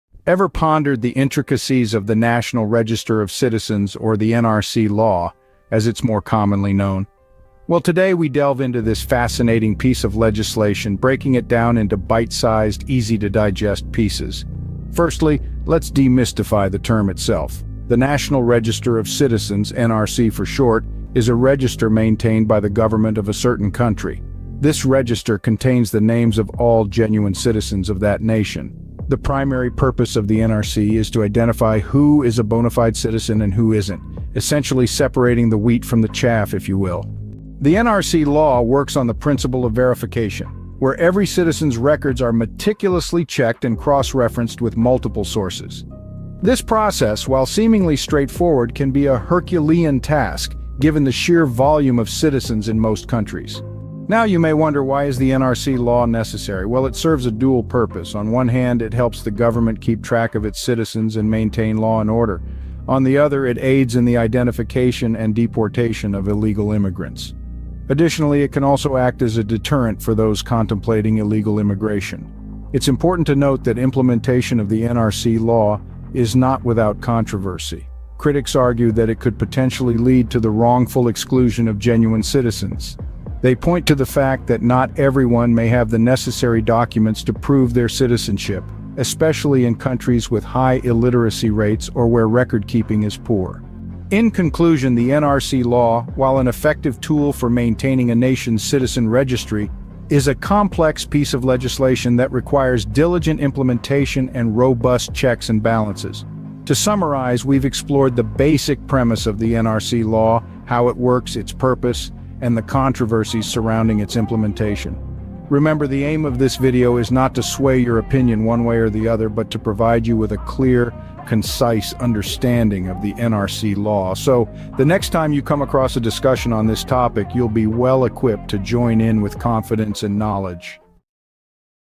Understanding NRC Law: Regulations, Implications, and Controversies || The World Journey
Explore the intricacies of the NRC law in India with insights on its nationwide implications, controversies, and the latest updates. Join us to delve into the complexities of the NRC process, its impact on citizenship, and the ongoing debates surrounding this significant legal framework.